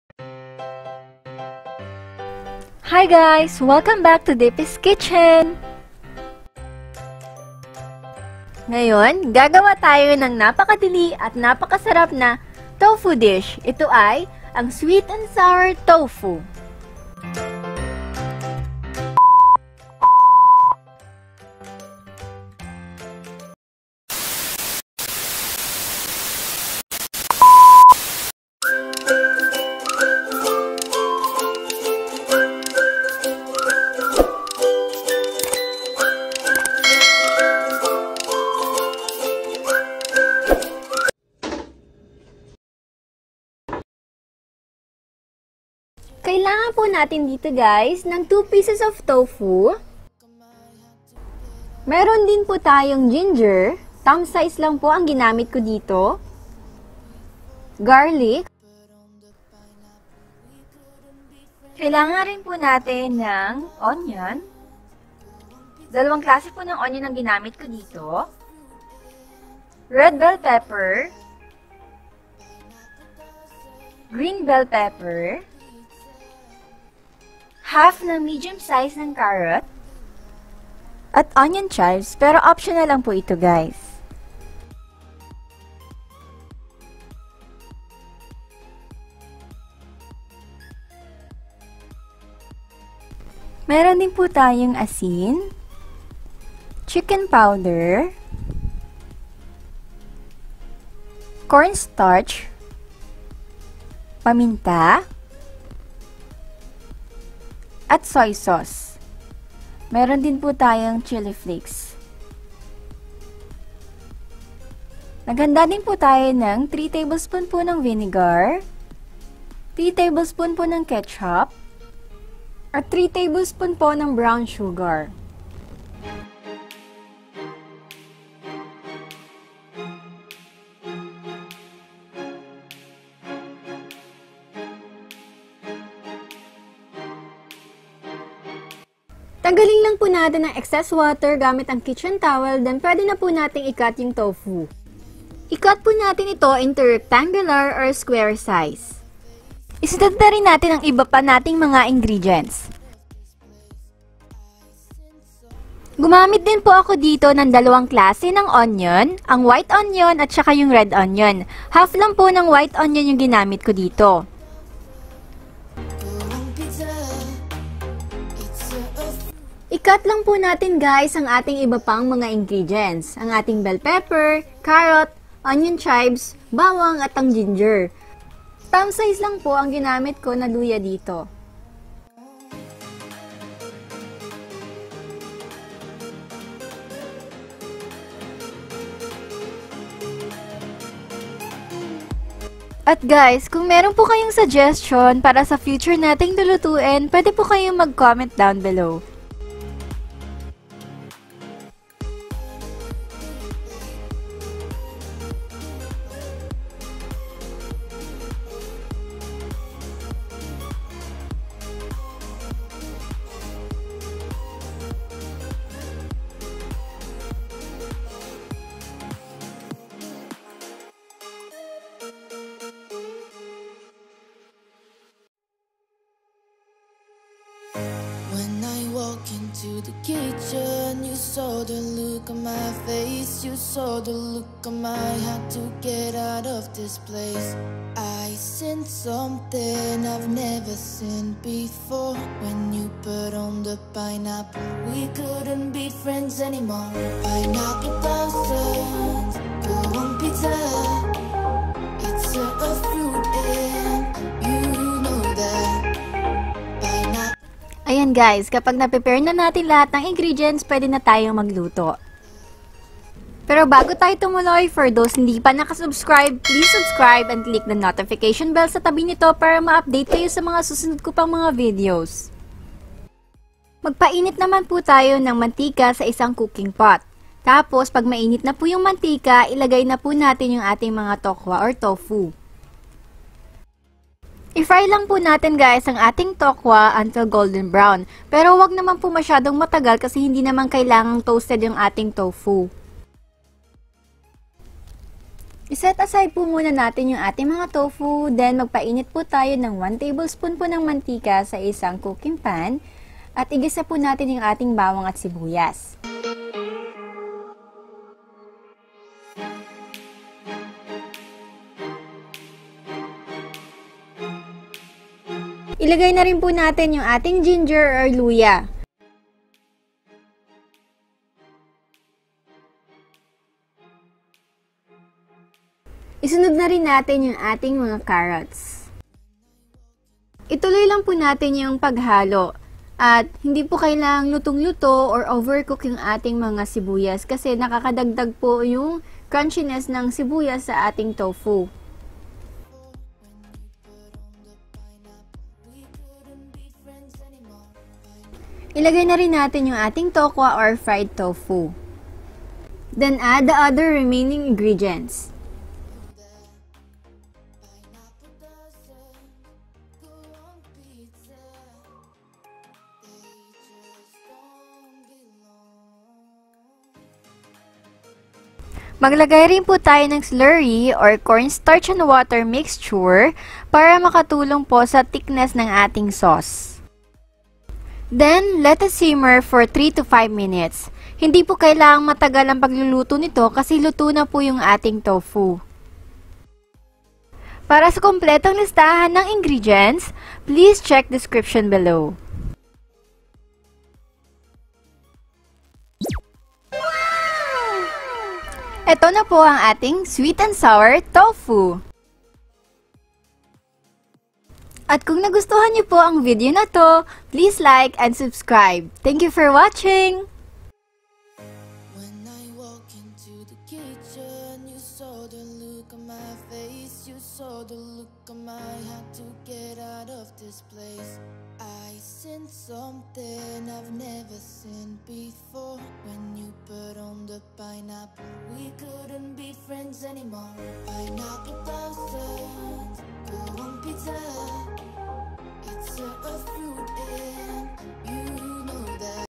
ᴳᴬᵂᴵᴺ ᴵᵀᴼ ˢᴬ ᵀᴼᴷᵂᴬ! ᴴᴼᵂ ᵀᴼ ᶜᴼᴼᴷ ˢᵂᴱᴱᵀ & ˢᴼᵁᴿ ᵀᴼᶠᵁ! ᵀᴼᶠᵁ ᴹᴬˢ ᴾᴬˢᴬᴿᴬᴾᴵᴺ ᴾᴬ! ᴴᴱᴬᴸᵀᴴʸ ᵁᴸᴬᴹ ᴺᴬ ᴬᴮᴼᵀ ᴷᴬʸᴬ!
ᴵⁿᵍʳᵉᵈⁱᵉⁿᵗs

² ᵖᶜˢ ᵒᶠ ᵗᵒᶠᵘ/ᵗᵒᵏʷᵃ
¹/² ʷʰⁱᵗᵉ ᵒⁿⁱᵒⁿ ⁽ᶜᵘᵗ ⁱⁿᵗᵒ ⁴ ᵒʳ ⁶ ᵖᶜˢ⁾
¹ ʳᵉᵈ ᵒⁿⁱᵒⁿ ⁽ᶜᵘᵗ ⁱⁿᵗᵒ ⁴ ᵖᶜˢ⁾
³ ᶜˡᵒᵛᵉˢ ᵒᶠ ᵍᵃʳˡⁱᶜ ⁽ᶜʰᵒᵖᵖᵉᵈ⁾
ᴼᴺᴵᴼᴺ ᶜᴴᴵⱽᴱˢ ⁽ᵒᵖᵗⁱᵒⁿᵃˡ⁾
¹/² ᵐᵉᵈⁱᵘᵐ ˢⁱᶻᵉ ᶜᵃʳʳᵒᵗ
¹ ᵗᵇˢᵖ. ᶜʰⁱᶜᵏᵉⁿ ᵖᵒʷᵈᵉʳ
¹/² ᵗˢᵖ ᶜʰⁱˡⁱ ᶠˡᵃᵏᵉˢ
ᴳⁱⁿᵍᵉʳ ⁽ᵗʰᵘᵐᵇ ˢⁱᶻᵉ⁾
³ ᵗᵇˢᵖ ᵇʳᵒʷⁿ ˢᵘᵍᵃʳ ⁽ʸᵒᵘ ᶜᵃⁿ ᵃᵈᵈ ¹ ᵗᵇˢᵖ ⁱᶠ ʸᵒᵘ ʷᵃⁿᵗ ᵐᵒʳᵉ ˢʷᵉᵉᵗ⁾
¹ ᵗᵇˢᵖ ˢᵒʸˢᵃᵘᶜᵉ
¹ ᵗᵇˢᵖ ᵒⁱˡ ᶠᵒʳ ˢᵃᵘᵗᵉⁱⁿᵍ

ˢˡᵘʳʳʸ ⁻ ² ᵗᵇˢᵖ ʷᵃᵗᵉʳ ᵖˡᵘˢ ¹ ᵗᵇˢᵖ ᶜᵒʳⁿˢᵗᵃʳᶜʰ

ₚᵣₑₚₐᵣₑ qᵤᵢcₖ & ₑₐₛᵢₗy ₘₑₐₗ fₒᵣ yₒᵤᵣ Fₐₘᵢₗy!                                                                                                                       ₚₗₛ. wₐₜcₕ ₜₕᵢₛ ᵥᵢdₑₒ & ₕₒₚₑ yₒᵤ ₗᵢₖₑ ᵢₜ!                                                                                                                                           ₜₕₐₙₖ Yₒᵤ!

₋dₒₙₙₐ ₚₑᵣₑz

sweet & sour tofu
how to cook Sweet and sour
healthy ulam
dp's kitchen and vlogs